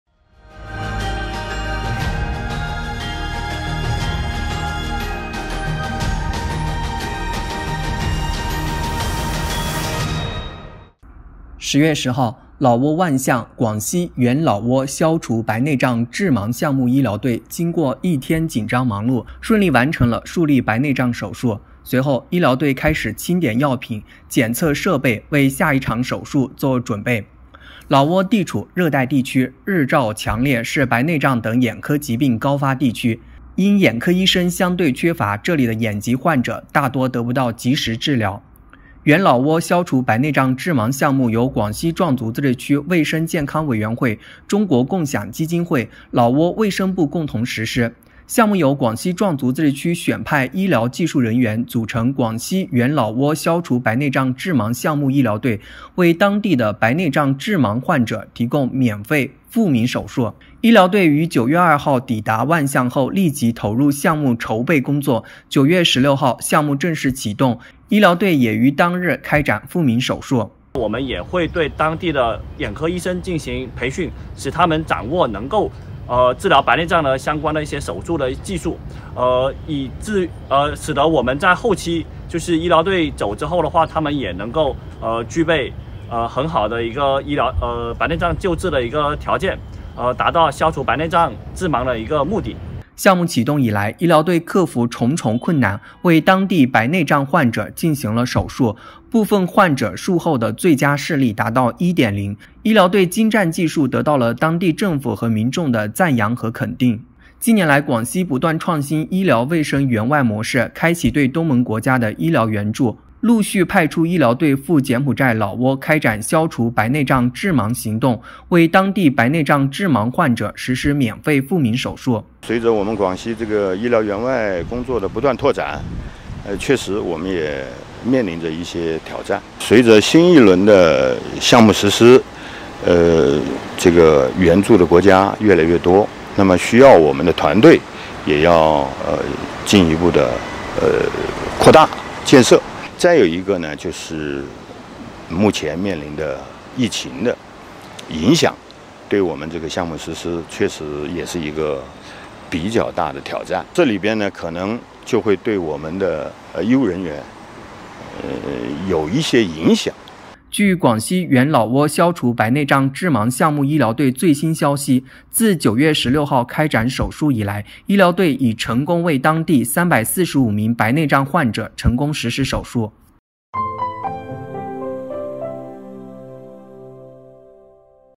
【澜湄印象】广西医疗队为老挝柬埔寨民众送“光明”
10月10日，老挝万象，广西援老挝消除白内障致盲项目医疗队经过一天紧张忙碌，顺利完成了数例白内障手术。随后，医疗队开始清点药品、检测设备，为下一场手术做准备。
老挝地处热带地区，日照强烈，是白内障等眼科疾病高发地区。因眼科医生相对缺乏，这里的眼疾患者大多得不到及时治疗。
援老挝消除白内障致盲项目由广西壮族自治区卫生健康委员会、中国共享基金会、老挝卫生部共同实施，项目由广西壮族自治区选派医疗技术人员组成广西援老挝消除白内障致盲项目医疗队，为当地的白内障致盲患者提供免费复明手术。